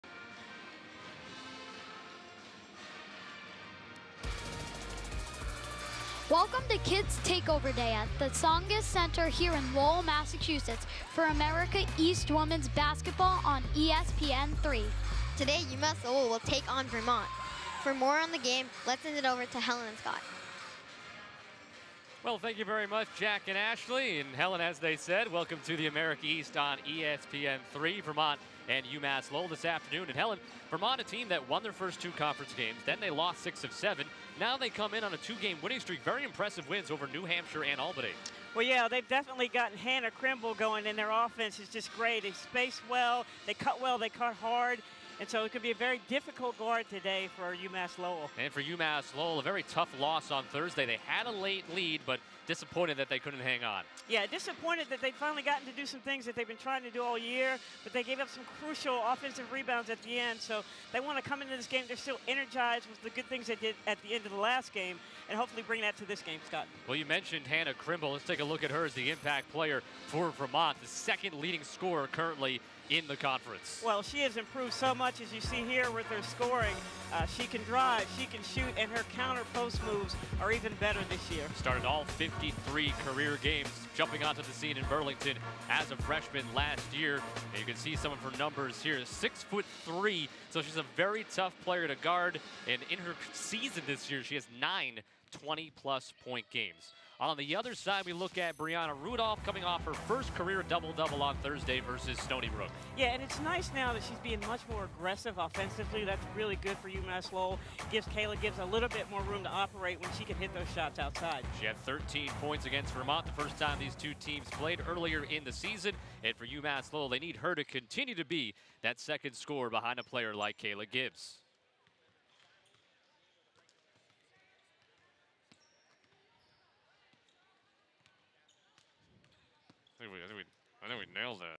ESPN3 Women's Basketball | UMass Lowell vs Vermont Open
Game open from ESPN3 broadcast on Kids Takeover Day, Feb. 11, 2018

Role: Producer